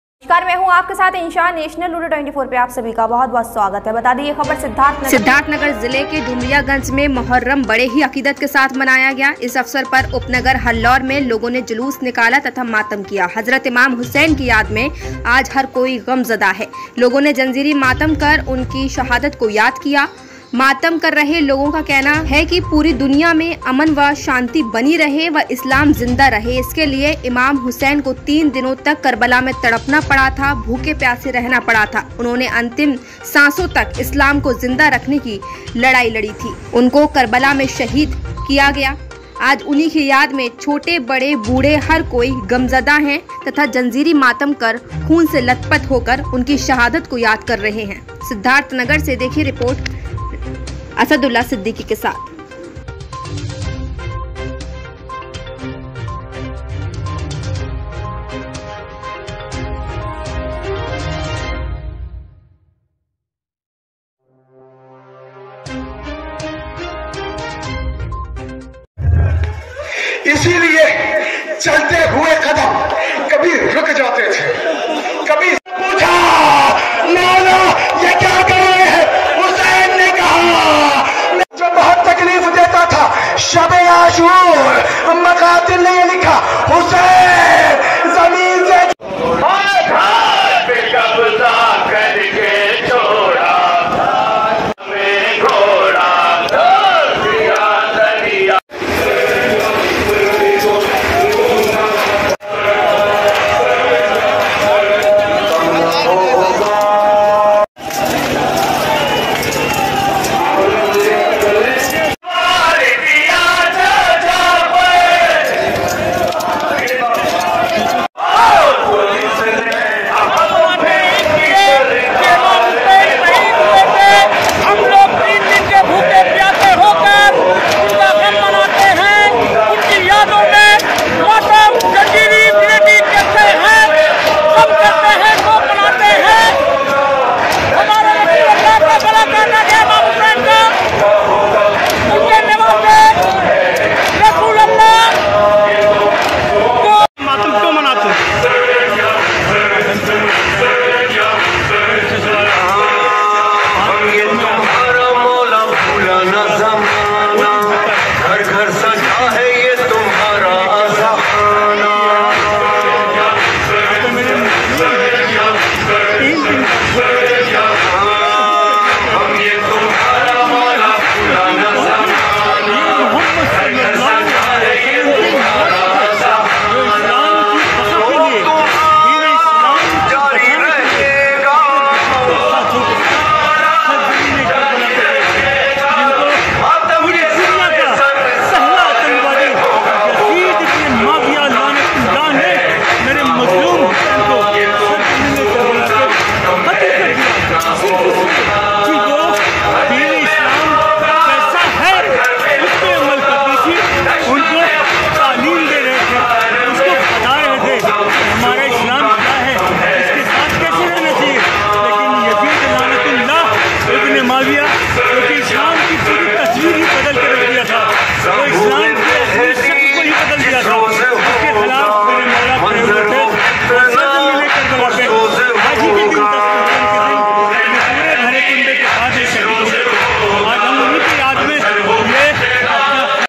डुमरियागंज में मोहर्रम बडे ही अकीदत के साथ मनाया गया इस अवसर पर उपनगर हल्लौर मे लोगो ने जुलूस निकाला
सिद्धार्थनगर जिले के डुमरियागंज में मोहर्रम बडे ही अकीदत के साथ मनाया गया इस अवसर पर उपनगर हल्लौर मे लोगो ने जुलूस निकाला।